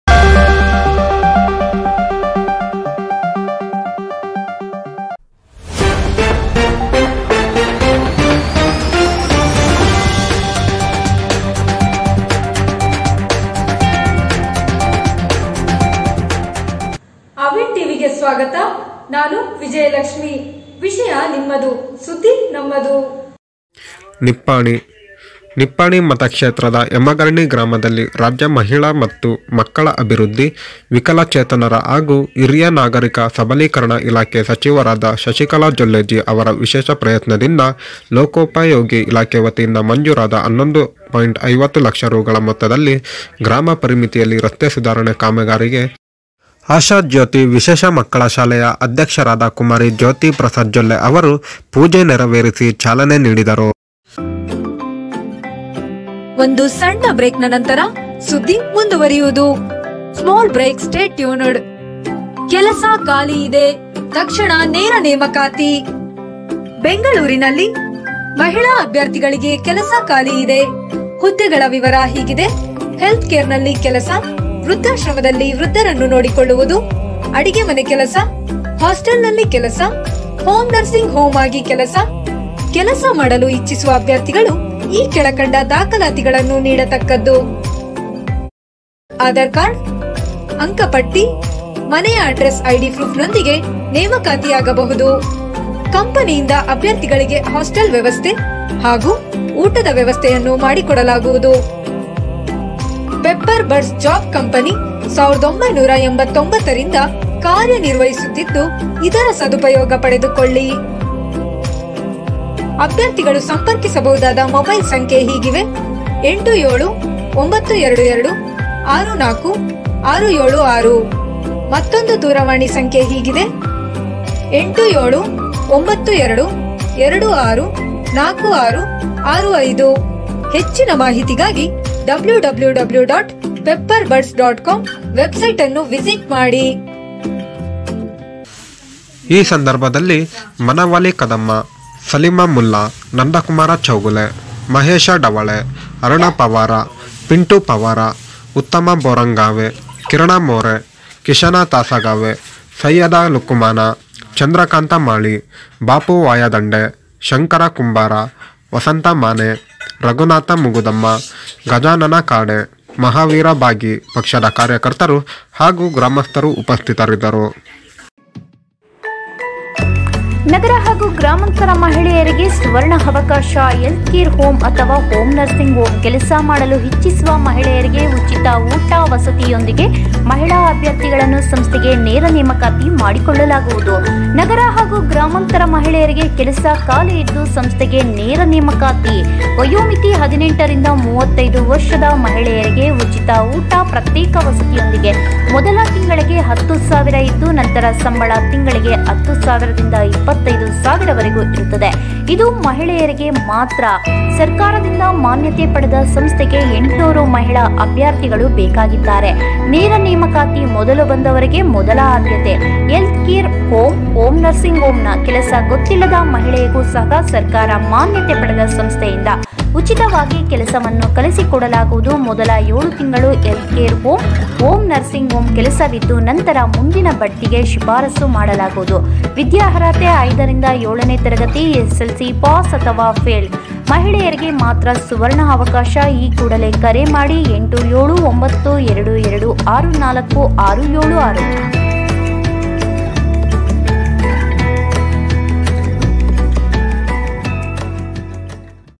ನಿಪ್ಪಾಣಿ ಮತಕ್ಷೇತ್ರದ ಯಮಗರ್ಣಿ ಗ್ರಾಮದಲ್ಲಿ ರಾಜ್ಯ ಮಹಿಳಾ ಮತ್ತು ಮಕ್ಕಳ ಅಭಿವೃದ್ಧಿ
ಟಿವಿ ಸುದ್ದಿ ವಾಹಿನಿಯಲ್ಲಿ ಕಾರ್ಯನಿರ್ವಹಿಸಲು ಜಿಲ್ಲಾ/ ತಾಲುಕಾ ಅನುಭವಿ ವರದಿಗಾರರ ಬೇಕಾಗಿದ್ದಾರೆ

ಸಂಪರ್ಕಿಸಿ

ಯಮಗರ್ಣಿ
ನಿಪ್ಪಾಣಿ ಮತಕ್ಷೇತ್ರದ ಯಮಗರ್ಣಿ ಗ್ರಾಮದಲ್ಲಿ ರಾಜ್ಯ ಮಹಿಳಾ ಮತ್ತು ಮಕ್ಕಳ ಅಭಿವೃದ್ಧಿ, ವಿಕಲಚೇತನರ ಹಾಗೂ ಹಿರಿಯ ನಾಗರಿಕರ ಸಬಲೀಕರಣ ಇಲಾಖೆ ಸಚಿವರಾದ ಸೌ. ಶಶಿಕಲಾ ಜೊಲ್ಲೆ ಜಿ, ಯವರ ವಿಶೇಷ ಪ್ರಯತ್ನದಿಂದ ಲೋಕೋಪಯೋಗಿ ಇಲಾಖೆವತಿಯಿಂದ ಮಂಜೂರಾದ 11.50 ಲಕ್ಷ ರೂಗಳ ಮೊತ್ತದಲ್ಲಿ ಗ್ರಾಮದ ಪರಿಮಿತಿಯಲ್ಲಿ ರಸ್ತೆ ಸುಧಾರಣೆ ಕಾಮಗಾರಿಗೆ ಆಶಾಜ್ಯೋತಿ ವಿಶೇಷ ಮಕ್ಕಳ ಶಾಲೆಯ ಅಧ್ಯಕ್ಷರಾದ ಕು. ಜ್ಯೋತಿಪ್ರಸಾದ ಜೊಲ್ಲೆ ಯವರು ಪೂಜೆ ನೆರವೇರಿಸಿ ಚಾಲನೆ ನೀಡಿದರು.
ಈ ಸಂದರ್ಭದಲ್ಲಿ ಶ್ರೀ. ವನಮಾಲಿ ಕದಮ, ಶ್ರೀ. ಸಲಿಮ ಮುಲ್ಲಾ, ಶ್ರೀ. ನಂದಕುಮಾರ ಚೌಗುಲೆ, ಶ್ರೀ. ಮಹೇಶ ಢವಳೆ, ಶ್ರೀ. ಅರುಣ ಪವಾರ, ಶ್ರೀ. ಪಿಂಟು ಪವಾರ, ಶ್ರೀ. ಉತ್ತಮ ಬೋರಗಾಂವೆ, ಶ್ರೀ. ಕಿರಣ ಮೋರೆ, ಶ್ರೀ. ಕಿಶನ ತಾಸಗಾವೆ, ಶ್ರೀ. ಸೈಯದ ಲುಕಮಾನ, ಶ್ರೀ. ಚಂದ್ರಕಾಂತ ಮಾಳಿ, ಶ್ರೀ. ಬಾಪು ವಾಯದಂಡೆ, ಶ್ರೀ. ಶಂಕರ ಕುಂಬಾರ, ಶ್ರೀ. ವಸಂತ ಮಾನೆ, ಶ್ರೀ. ರಘುನಾಥ ಮಗದುಮ್ಮ, ಶ್ರೀ. ಗಜಾನನ ಖಾಡೆ, ಶ್ರೀ. ಮಹಾವೀರ ಬಾಗಿ ಪಕ್ಷದ ಕಾರ್ಯಕರ್ತರು ಹಾಗೂ ಗ್ರಾಮಸ್ಥರು ಉಪಸ್ಥಿತರಿದ್ದರು.
निपाणी मतदारसंघातील यमगर्णी गावात राज्य महिला व बालविकास विभाग,अपंगत्व आणि ज्येष्ठ नागरिकांचे सशक्तिकरण विभागाच्या मंत्री सौ. शशिकला जोल्ले जी(वहिनी) यांच्या विशेष प्रयत्नातून सार्वजनिक बांधकाम विभाग कडून मंजूर झालेल्या 11.50 लाख रुपयांमध्ये गावाच्या हद्दीतील रस्ता सुधारणा कामाचे आशाज्योति विशेष मुलांच्या शाळेचे अध्यक्ष कु.ज्योतीप्रसाद जोल्ले यांनी पूजन करून चालना दिले.
यावेळी श्री. वनमाली कदम, श्री. सलीम मुल्ला, श्री. नंदकुमार चौगुले, श्री. महेश ढवळे, श्री. अरुण पवार, श्री. पिंटू पवार, श्री. उत्तम बोरगांवे, श्री. किरण मोरे, श्री. किशन तासगावे, श्री. सैयद लुकमान, श्री. चंद्रकांत माळी, श्री. बापु वायदंडे, श्री. शंकर कुंभार, श्री. वसंत माने, श्री. रघुनाथ मगदूम, श्री. गजानन खाडे, श्री. महावीर बागी पक्षाचे कार्यकर्ते आणि ग्रामस्थ उपस्थित होते.